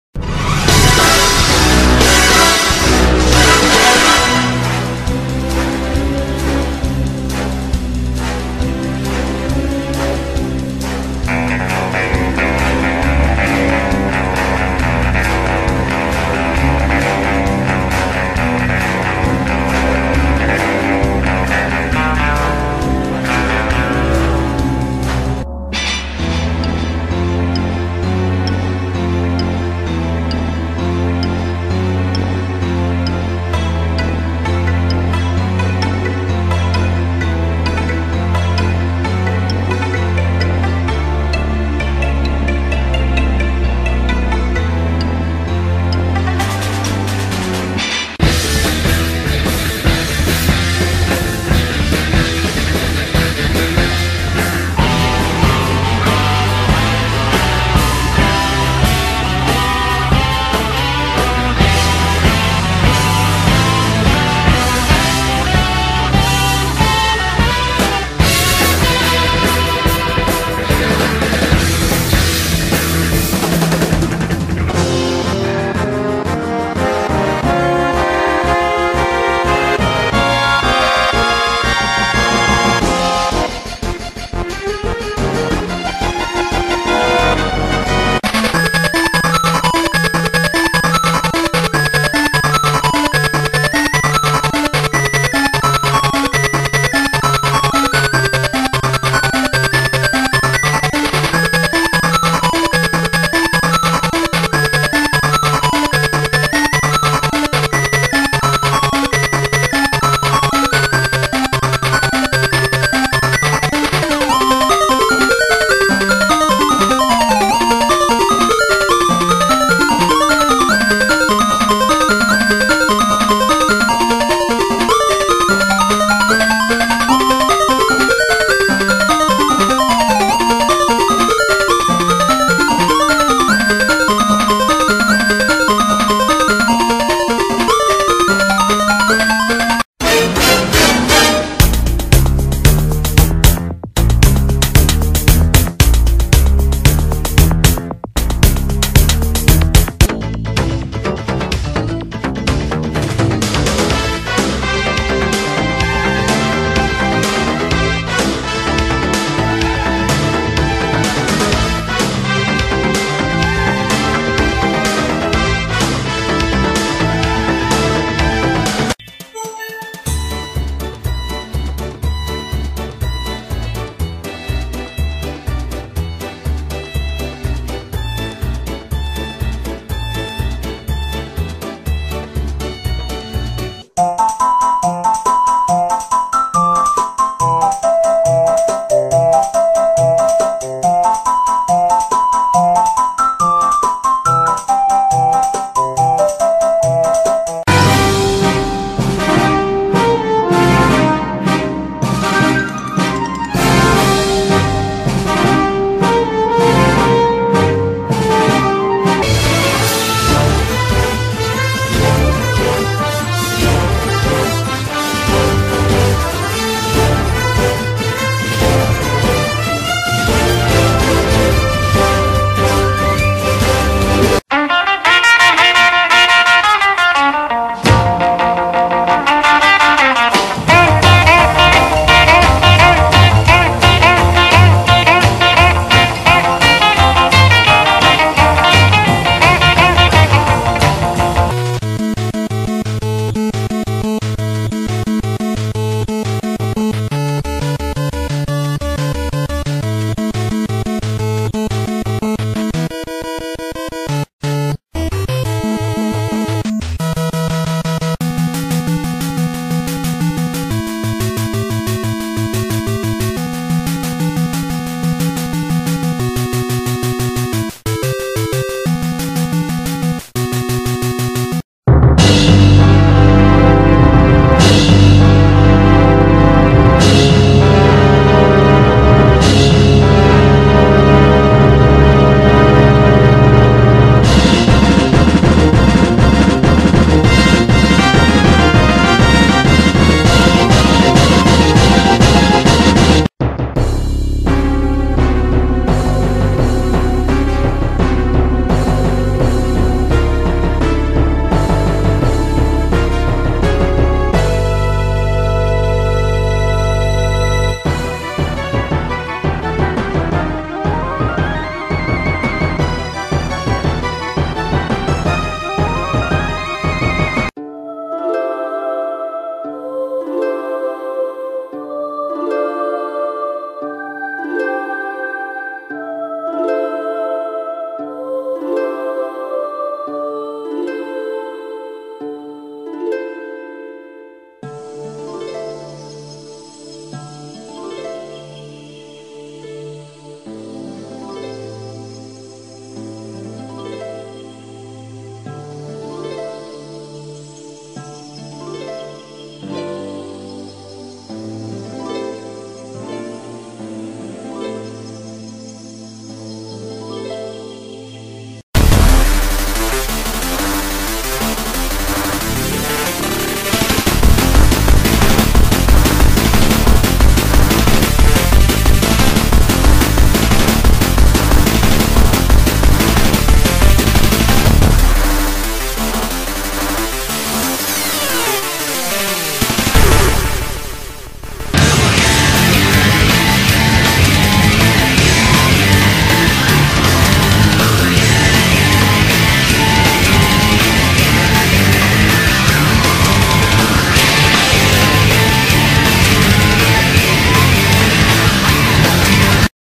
Familiar sounding video game themes and others 4
Video game themes and others that sound familiar Episode 4

- Some may sound just a bit like another tune
- Some may sound A LOT like another tune
- Some may be intentionally taken from another tune (same artist etc)

Its all in good fun, no need to take it too seriously.